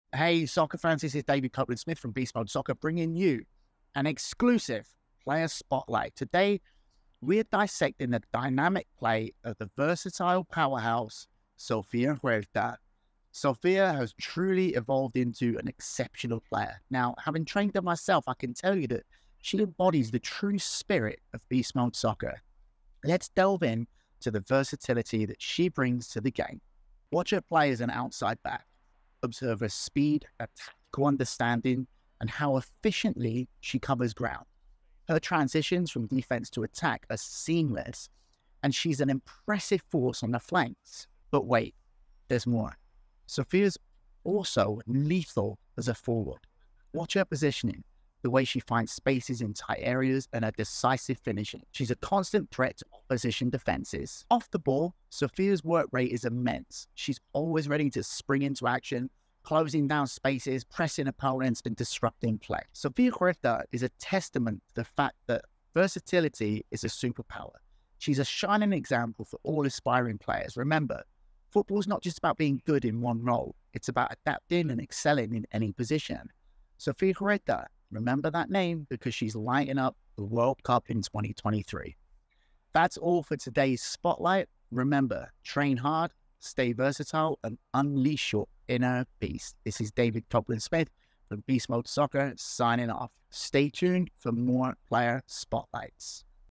World Cup Player Spotlight: Sofia Huerta
🔥PLAYER SPOTLIGHT ALERT🔥 Meet Sofia Huerta, the versatile powerhouse making waves at the Women's World Cup 2023! 🏆 I had the privilege of training Sofia and watching her transform into an absolute dynamo on the field. 💪 From defensive prowess to lethal striking, she's got it all! 🚀

Do you want to adapt and excel like Sofia? Then this series is for YOU! Introducing 🌟 'Beasts of the Pitch' 🌟 - your exclusive insider look into the journey and techniques of the players I've personally trained, all lighting up the World Cup! 💯🔥

Dive in, analyze, learn, and be inspired to unleash your inner BEAST!

P.S. - TAG a soccer-loving friend who needs to see this! ⚽️👀